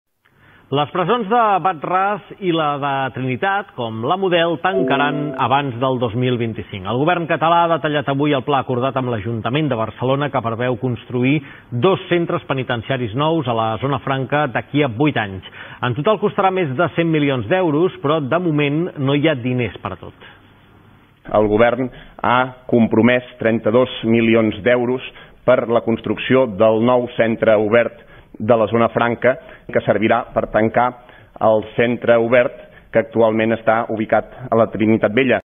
Les presons de Wad-Ras, la Trinitat i la Model tancaran abans del 2025
Tancament de les presons Model, WadRas i Trinitat
8TV. 16 de febrer de 2017